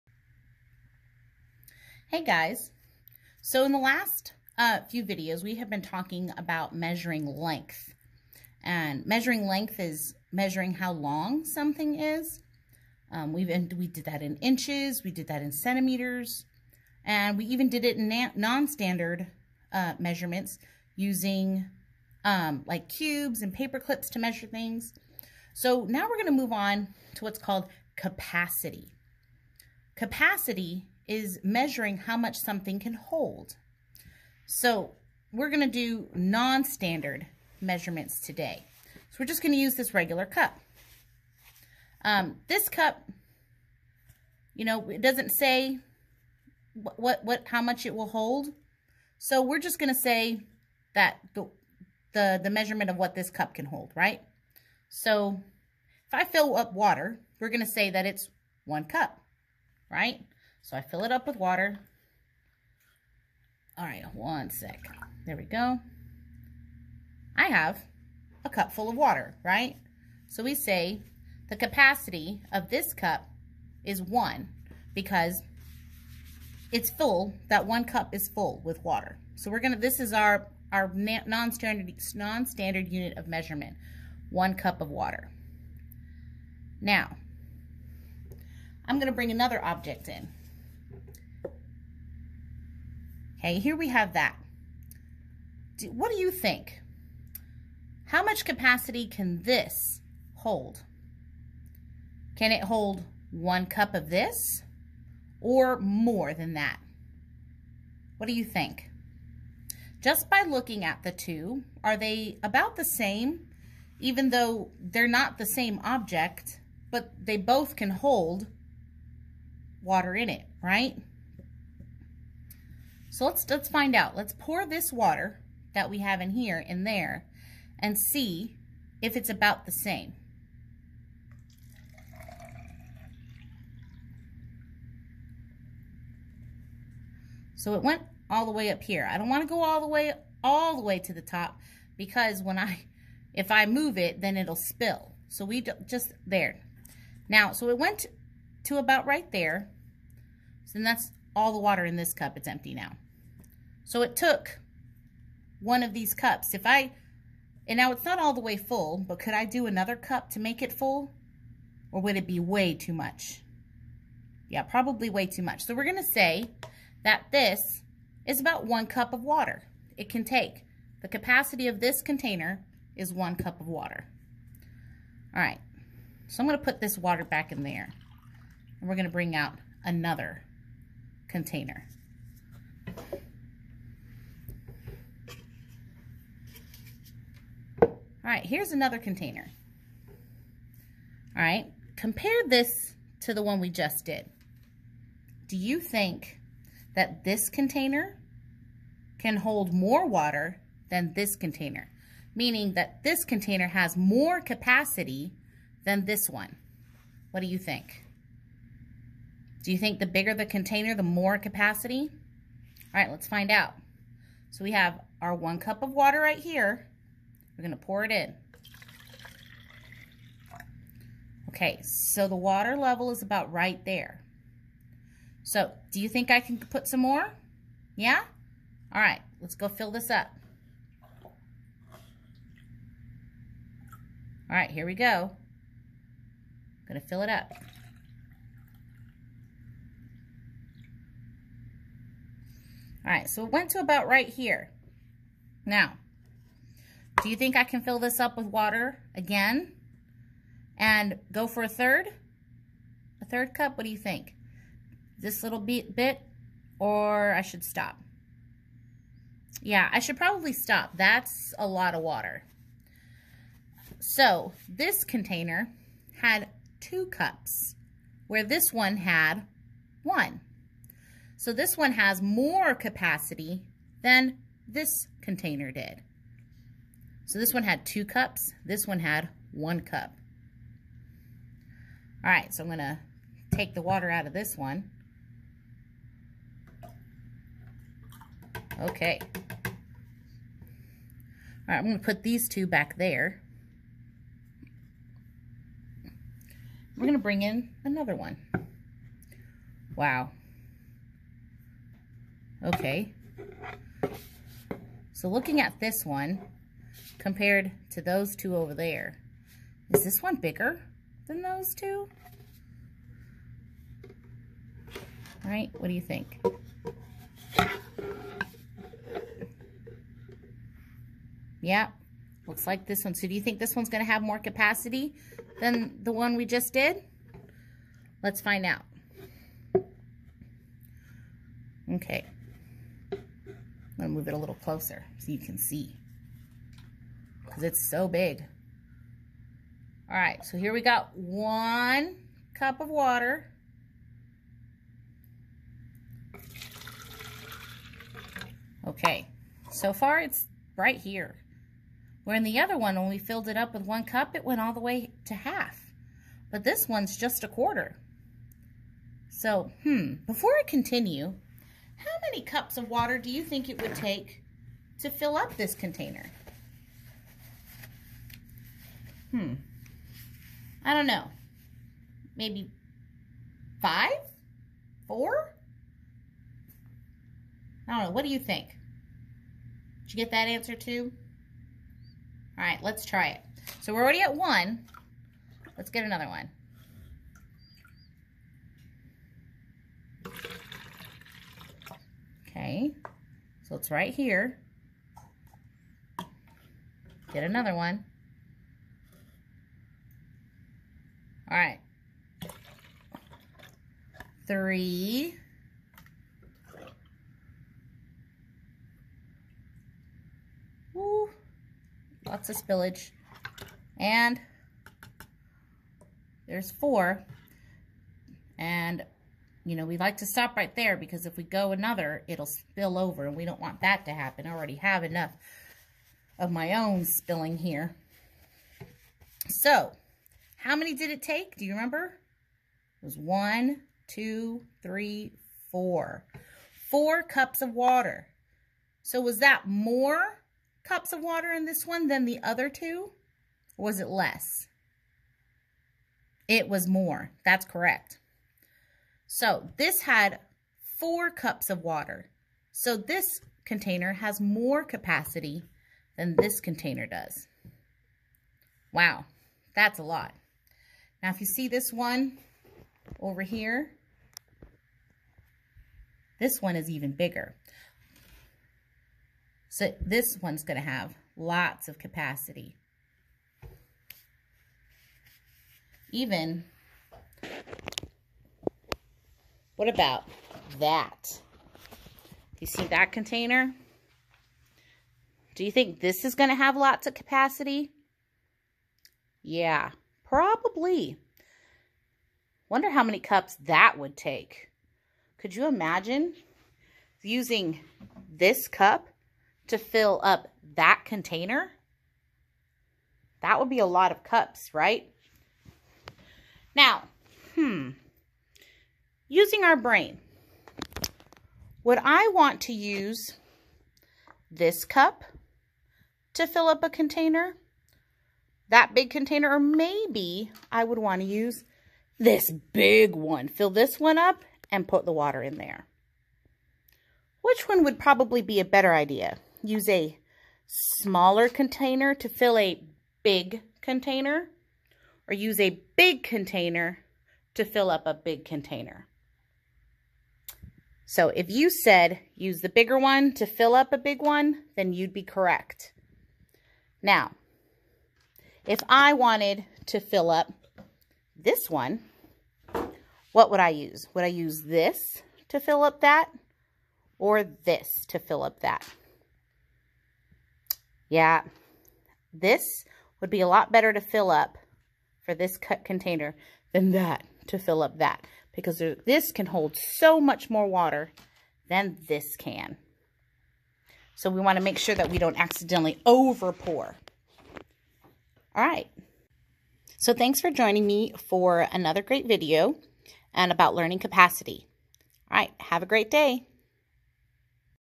Non-Standard Measurement-Capacity
How can we measure how much something can hold? In this video we will use a regular paper cup to measure capacity.